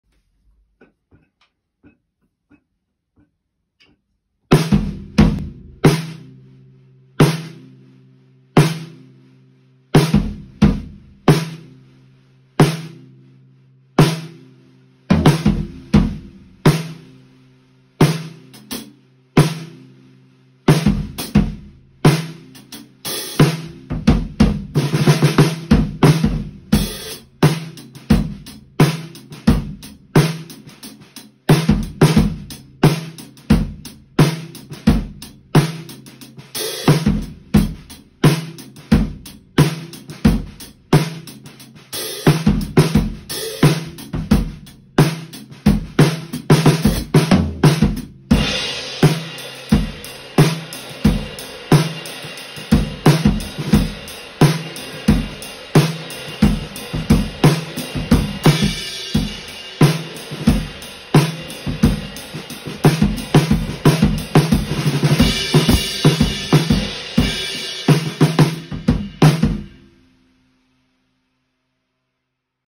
Pearl World Series Demo 1 - Baseball Bat
I have this snare up for sale! Please check out the other videos to hear it with some other tunings. This is my best attempt at the ‘baseball bat in a wedding cake’ type sound. Kick and snare are close mic’d, along with the x/y overhead pair plus a stereo room mic. Processed this afterwards in Logic using mainly stock plugins.